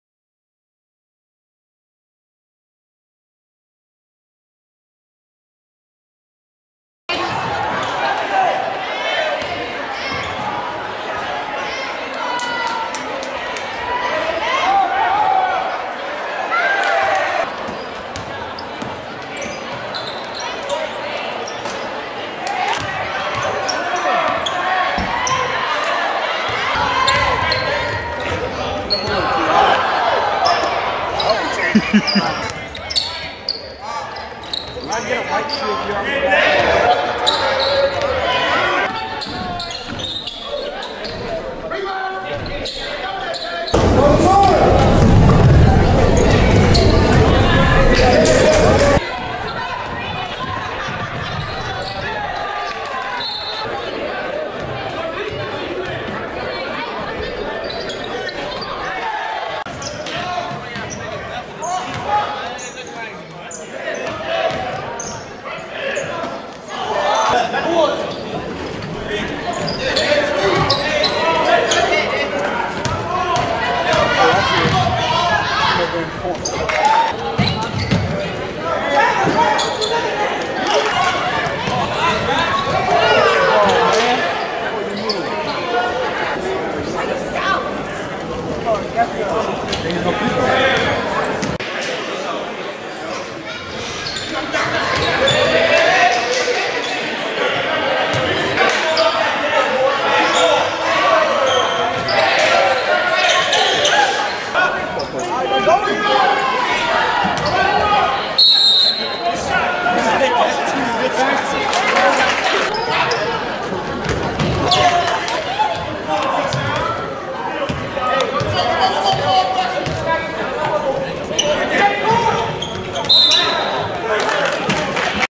David Okeke #21 C/O 2017 DuVal High School 2014-2015 Season Highlights
#21 David Okeke 6'5 190 Lbs  Small Forward, Power Forward, Center